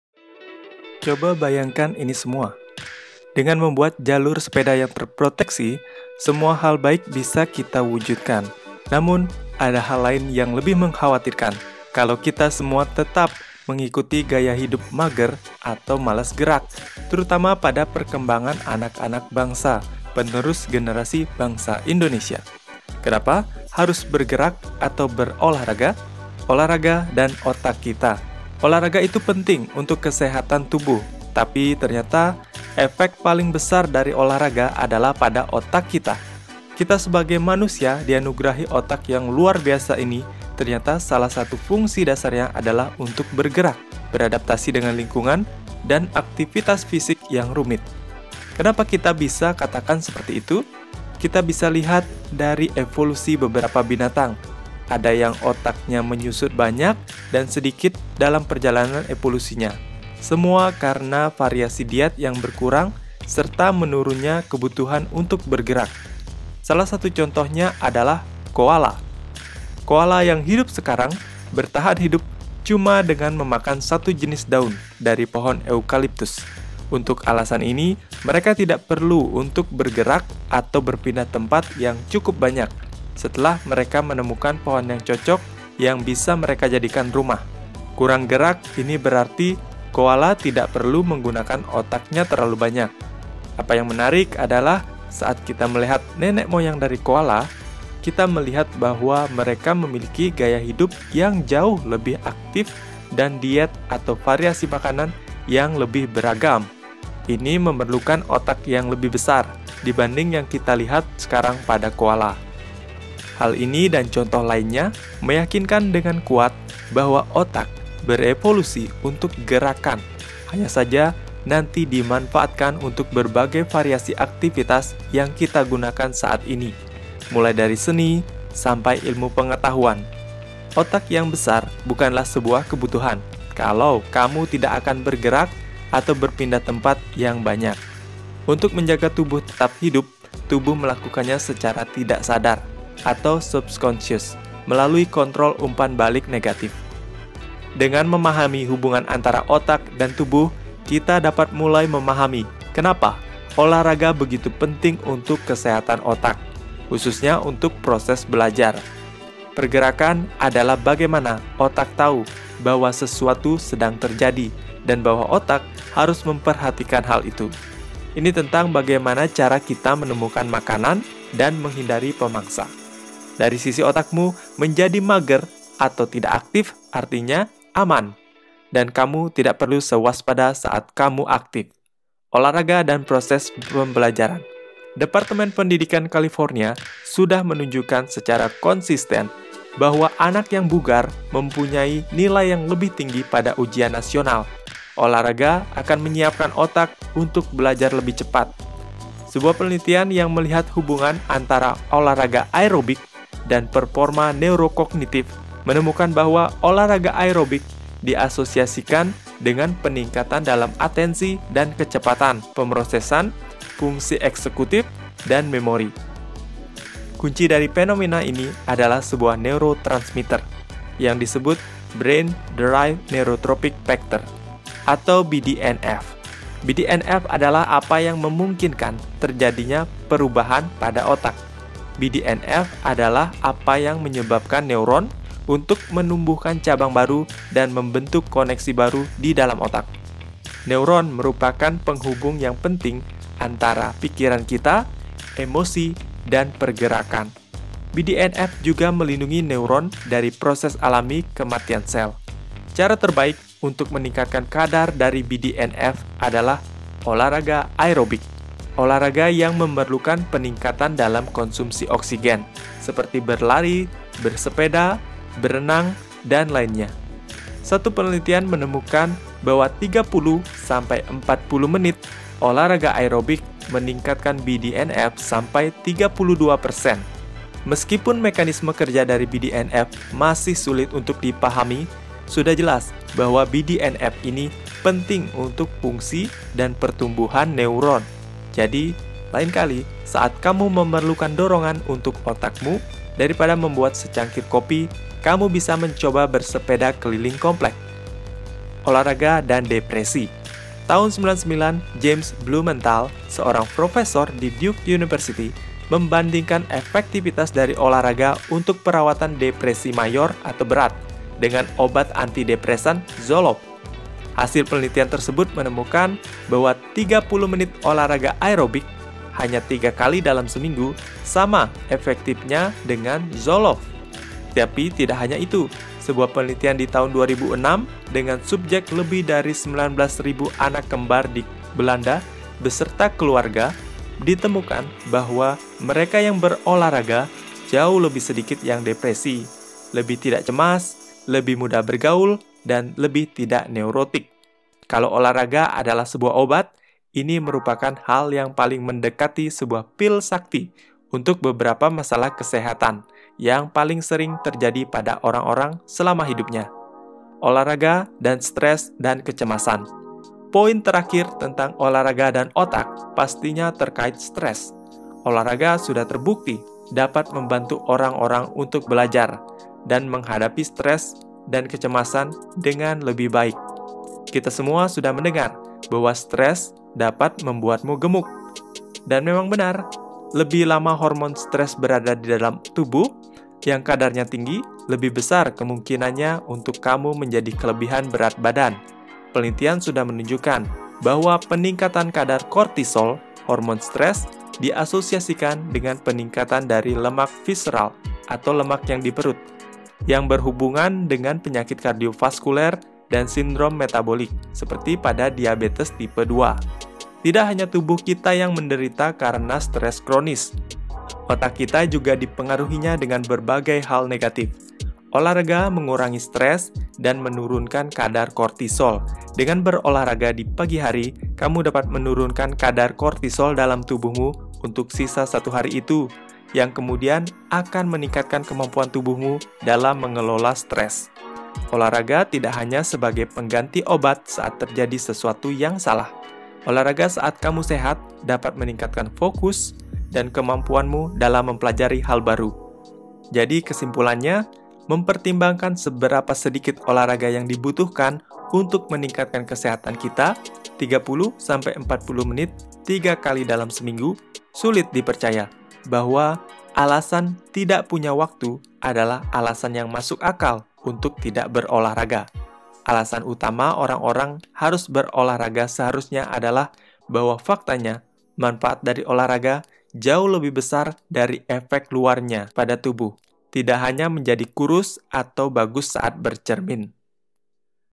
WAJIB TAU - BaliBersepeda Make Bali Cycling Paradise - Part 1
Sudah saatnya kita memulai revolusi transportasi untuk kebaikan bersama

BaliBersepeda.com